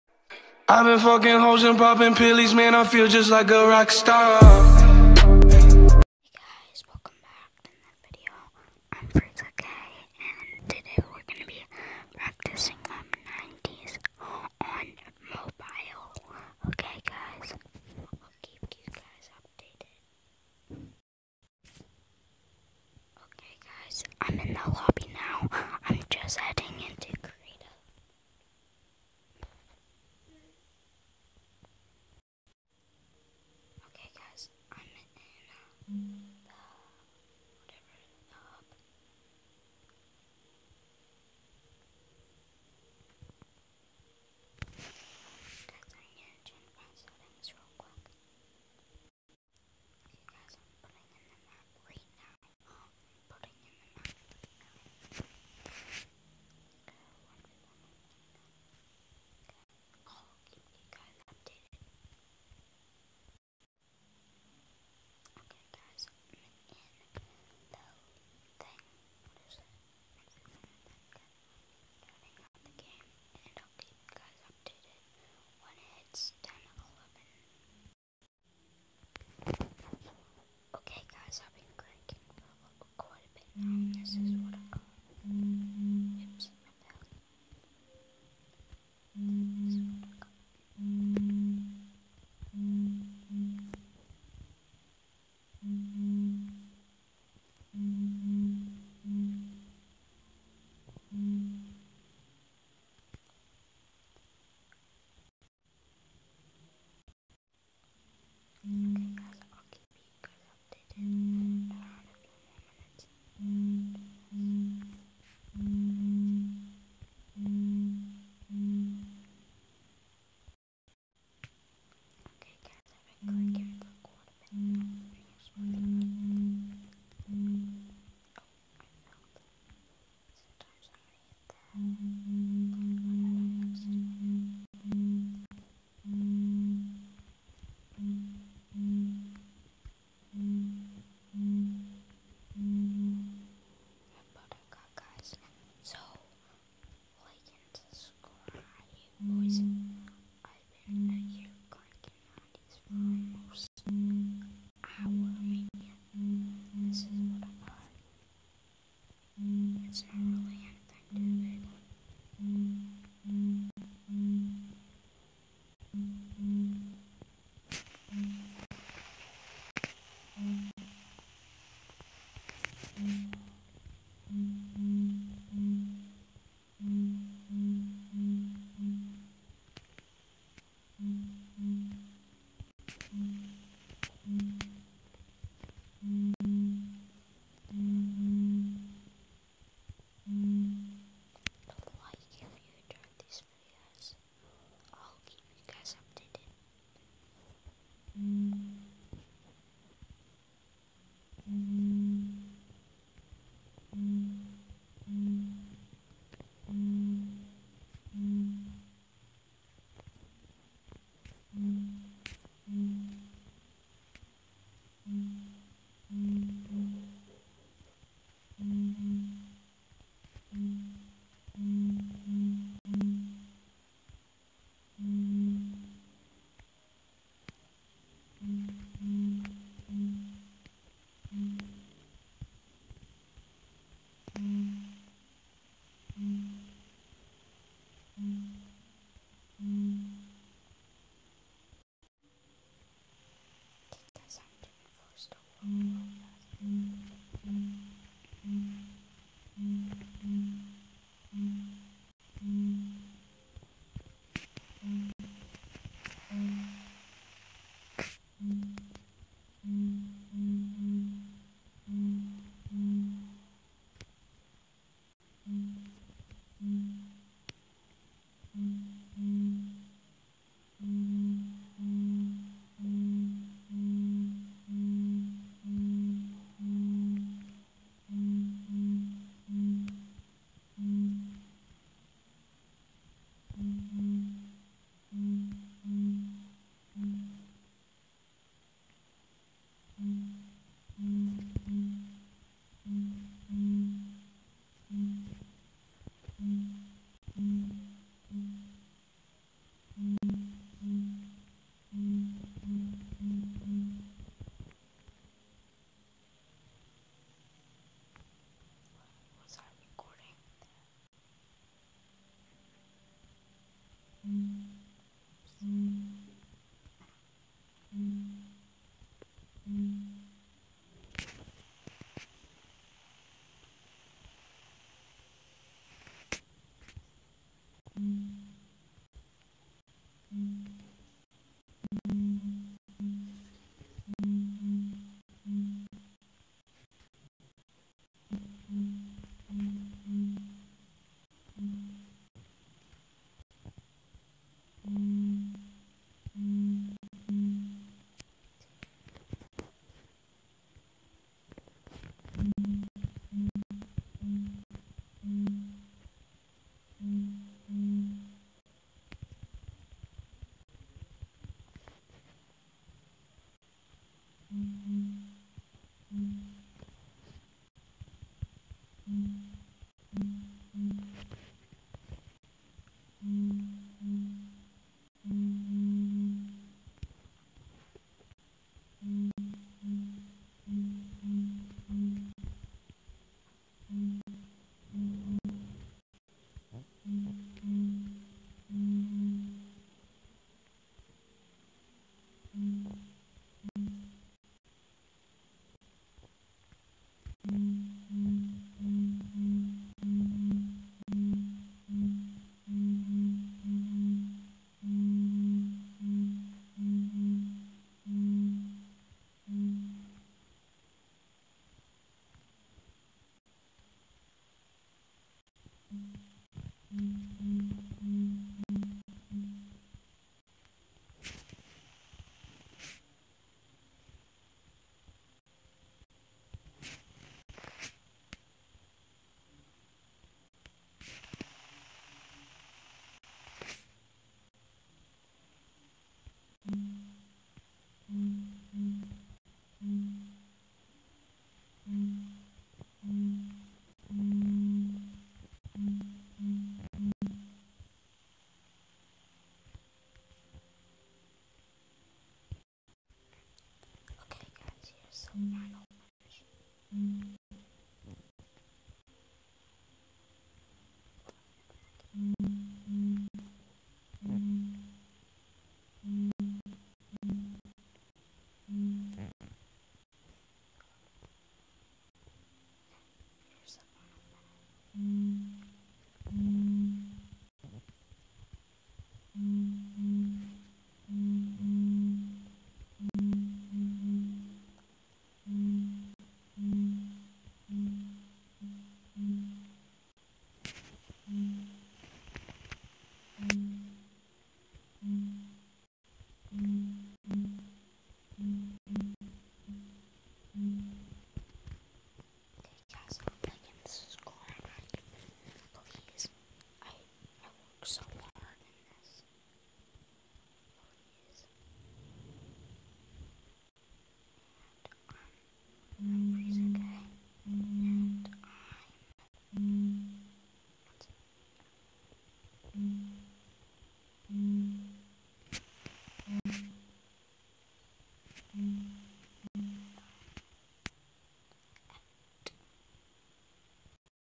Practicing my building on mobile
This is the first time I have crank 90s I am so bad